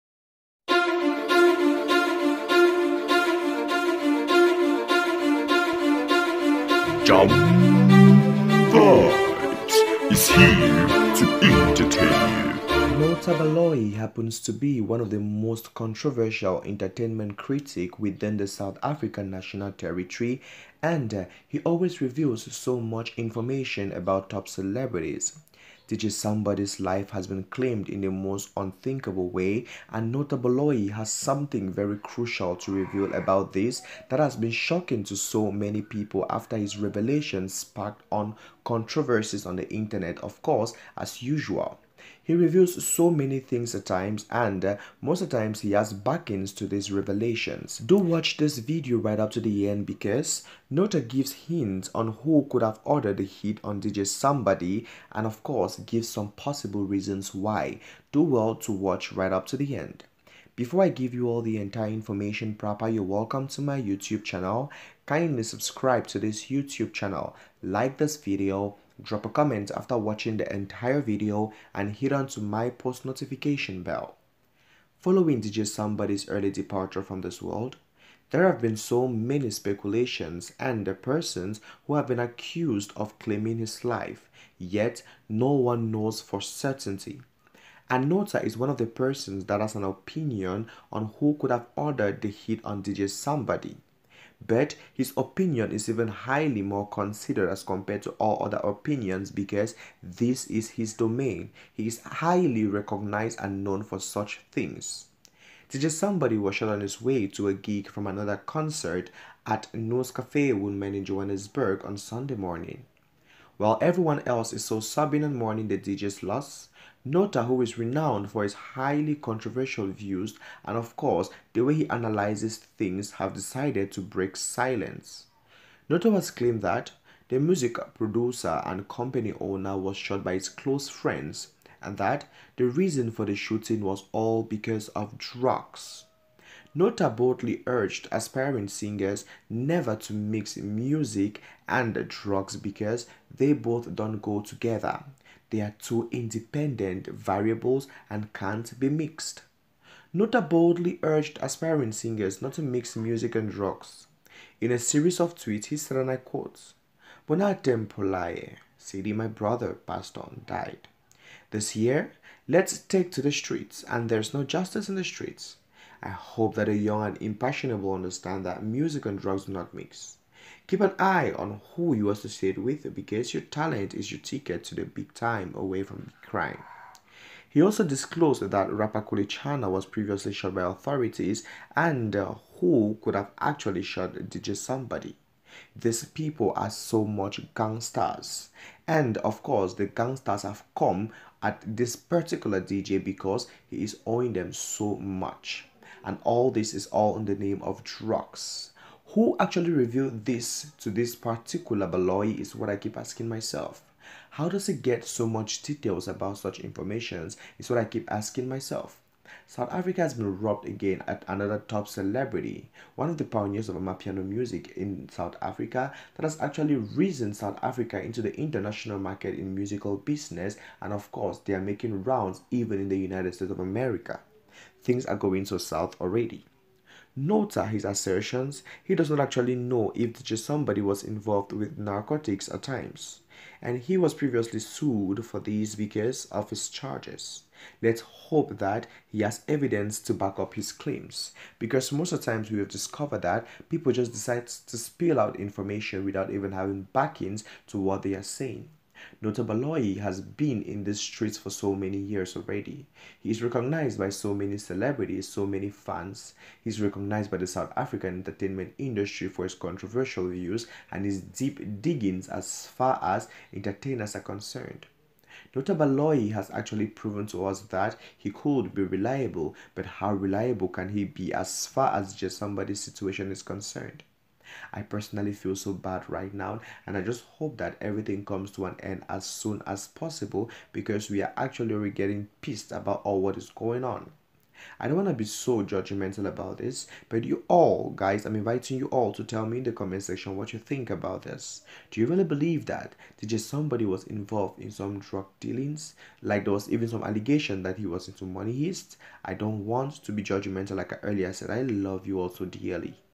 DJ Sumbody's K!llers are Exposed, See why they K!lled him
DJ Sumbody's K!llers are Exposed, See why they K!lled him according to Nota Baloyi